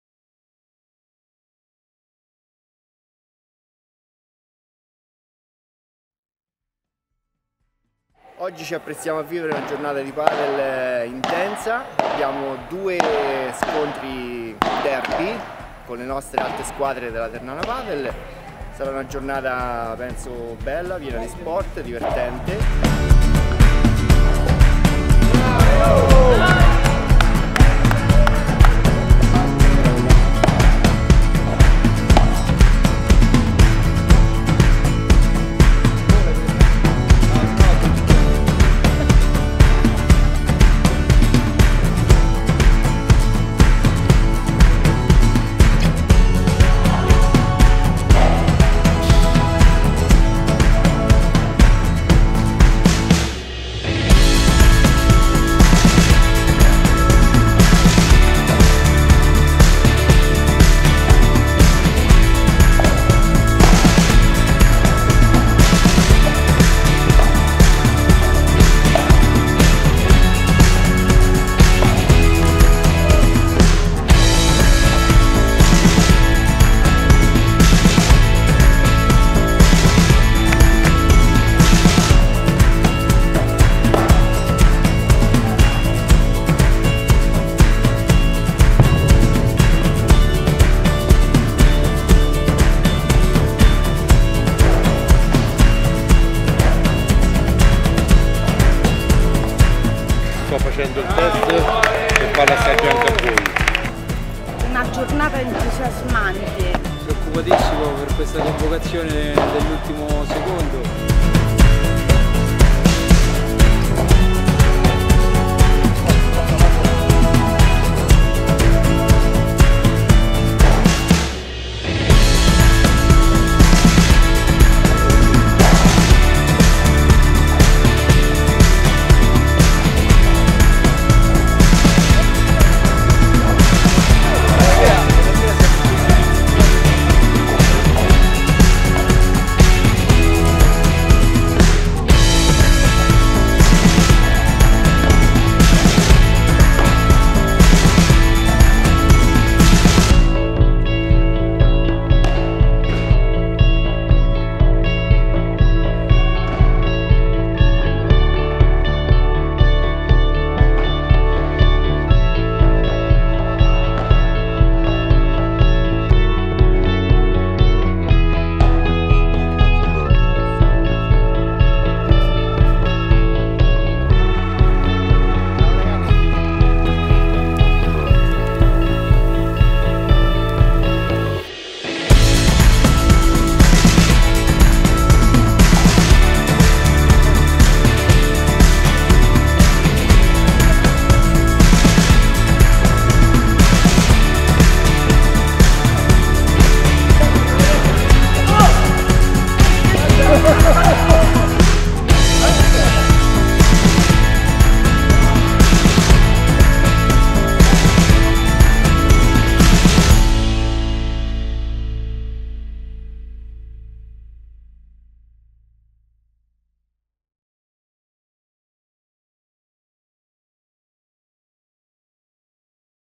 Derby MSP alla Ternana Padel
Ieri è stata una domenica di padel. Ma questo avviene alla TP ogni fine settimana. E' stata una domenica di divertimento all'insegna dello stare insieme. Ma pure questo avviene alla TP ogni domenica... Ieri è stata una domenica di derby, e questo invece è piuttosto raro. Tutte e quattro le squadre MSP Umbria Sport Fun rossoverdi si sono scontrate in un derby all'ultima bandeja, e oltre a combattere sui campi, divertirsi, commentare, ridere e stare insieme a bordocampo, abbiamo deciso di celebrare il legame stretto che lega tutti i membri del nostro circolo, dal presidente fino all'ultimo arrivato festeggiando come meglio sappiamo fare, mangiando, stando insieme, divertendoci e prendendoci in giro. Ed è stato un successo. Ecco un piccolo documento video che racconta questa bella giornata.